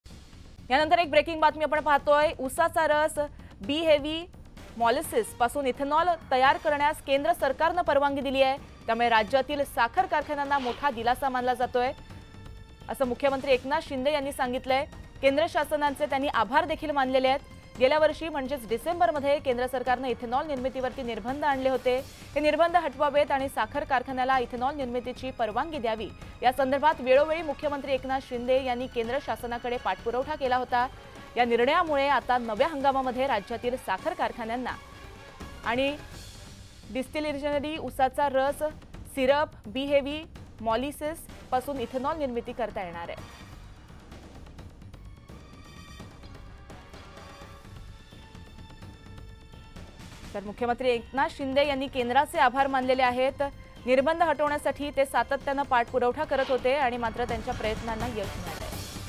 CM Eknath Shinde On Central Government | इथेनॉल निर्मितीस परवानगी,राज्यातील साखर कारखान्यांना दिलासा
The central government has allowed the production of ethanol from sugarcane juice, B-heavy molasses. Chief Minister Eknath Shinde thanked the central government saying that the sugar mills of the state have got a big relief due to this. Last year i.e. in December, the central government had imposed restrictions on the production of ethanol. From time to time Chief Minister Shri. Shinde had followed up with the central government.
Due to this decision, sugar factories and distilleries in the state will now be able to produce ethanol from sugarcane juice, syrup, B heavy molasses in the new season.

उसाचा रस, बी-हेवी मेालॅसिस पासून इथेनॉल तयार करण्यास केंद्र सरकारने परवानगी दिलीय. त्यामुळं राज्यातील साखर कारखान्यांना मोठा दिलासा मिळाला असल्याचं सांगत मुख्यमंत्री एकनाथ शिंदे यांनी केंद्र शासनाचे आभार मानलेत. गेल्या वर्षी म्हणजे डिसेंबरमध्ये केंद्र सरकारने इथेनॉल निर्मितीवर निर्बंध आणले होते. हे निर्बंध हटवावेत आणि साखर कारखान्यांना इथेनॉल निर्मितीची परवानगी द्यावी यासंदर्भात वेळोवेळी मुख्यमंत्री श्री. शिंदे यांनी केंद्र शासनाकडे पाठपुरावा केला होता.
या निर्णयामुळे आता नव्या हंगामात राज्यातील साखर कारखान्यांना आणि डिस्टिलरीजना उसाचा रस, सिरप, बी हेवी मेालॅसिस पासून इथेनॉल निर्मिती करता येणार आहे.

UTNA